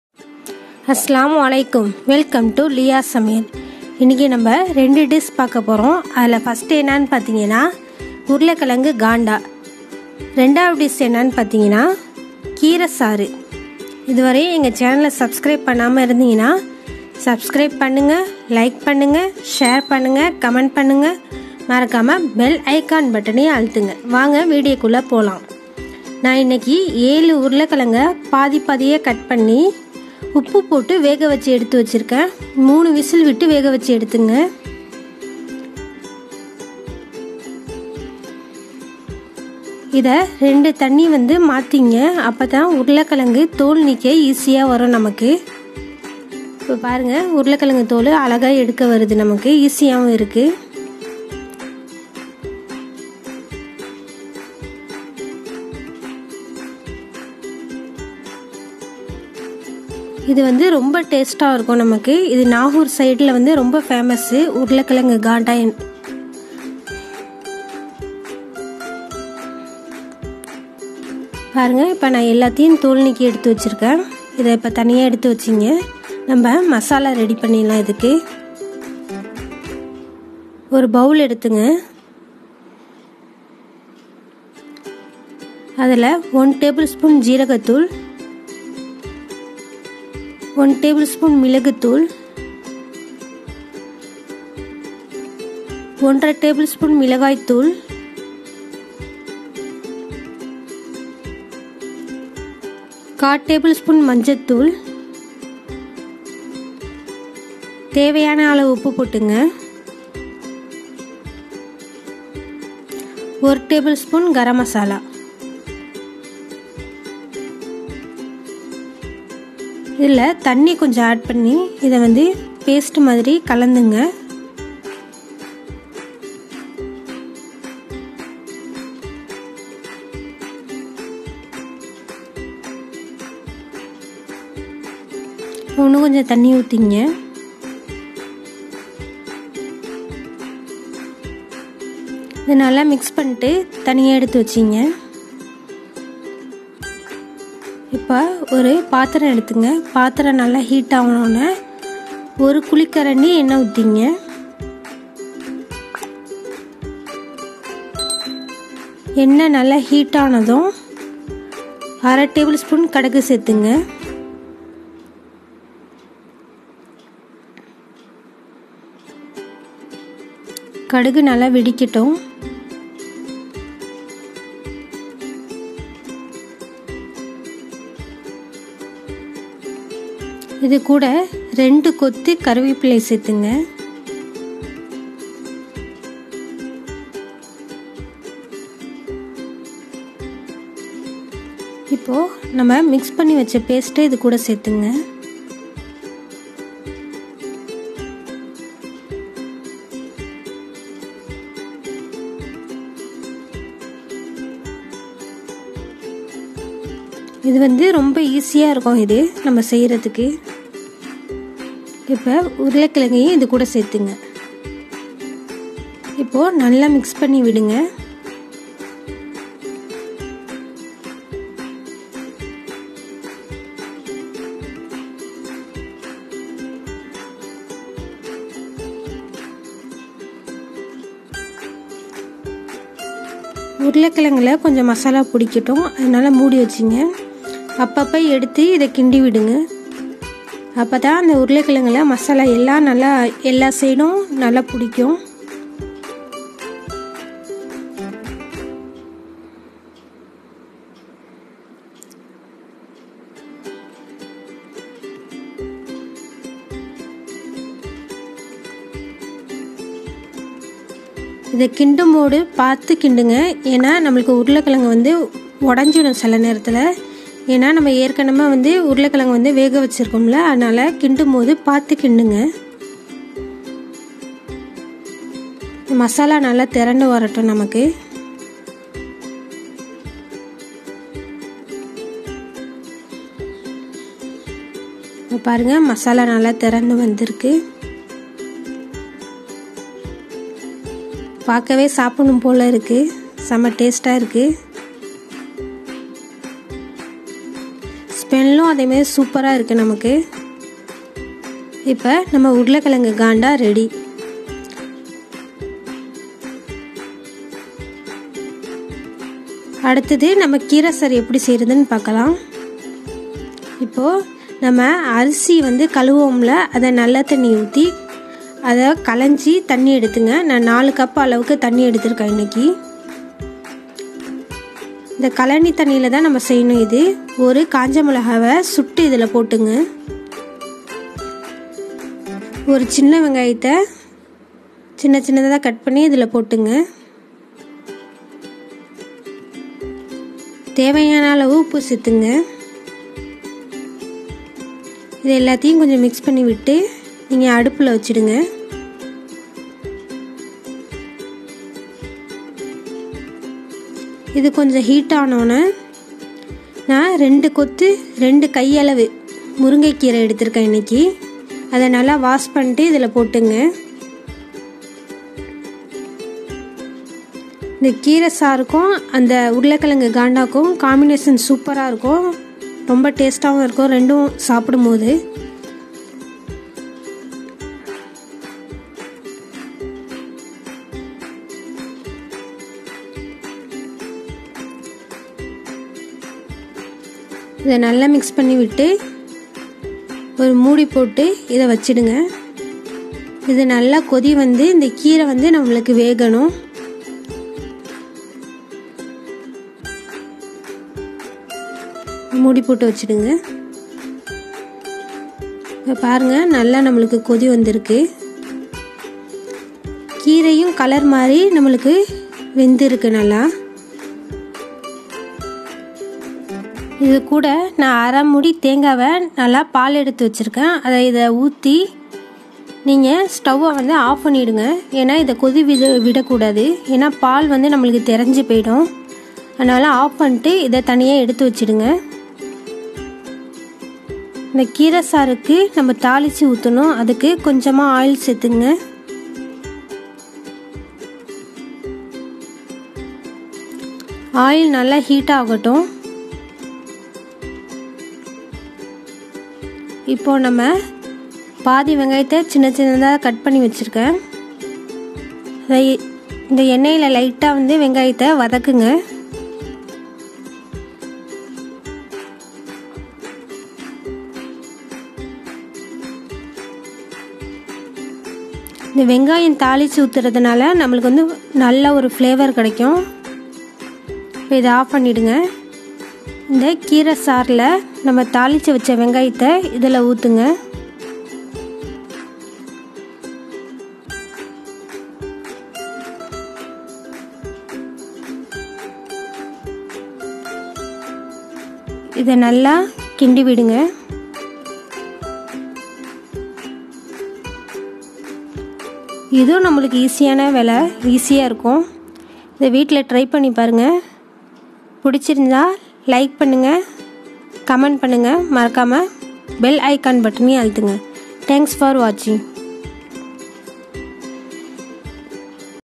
Nagore Special Lunch: Potato Ganda with Spinach Soup | நாகூர் ஸ்பெஷல் காண்டா | முருங்கை கீரை சாறு
Friends, this video is about our family lunch where I prepared local dishes. Nagore special potato Ganda with spinach soup is what I am going to show you today.

Potato Ganda is famous in nagore and surroundings which is made spicy of boiled potatoes. Normally it is had as evening snack in roadside shops. It is also suited for rice.

Spinach soup is made with drumstick leaves which is very healthy and easy to make.

Let me describe the preparation.

INGREDIENTS

Potato Ganda:

Potato - 7
Cummin powder - 1 tbsp
Pepper - 1 tbsp
Chilly powder - 1.5 tbsp

Spinach soup:

Drumstick leaves - 2 handful
Rice broth - 4 cup
Coconut milk - 1/2 coconut
Oil - little

Preparation: Potato Ganda

1. Cut the potato into halves
2. Boil the same with salt for 3 whistles
3. Peel the skin and keep aside
4. Mix all the masalas in a bowl with water to a paste
5. In a kadai, add oil
6. Once hot, add mustard and let it fry
7. Now add curry leaves and masala mix
8. Let it cook
9. Now add boiled potatoes and let it cook until dry

Preparation: Keerai Saaru (Spinach Soup)

1. Take rice cleaned water
2. To it add chopped onion and red chilly
3. Add salt to taste
4. Let it boil
5. Now add spinach to it and allow it to cook
6. Now add coconut milk and switch off the flame
7. Fry chopped onion with oil in a kadai
8. Add it to the soup and serve it hot

Friends, please do try this recipes at home. Do share your feedback about the recipe in the comments below. All the best and happy cooking!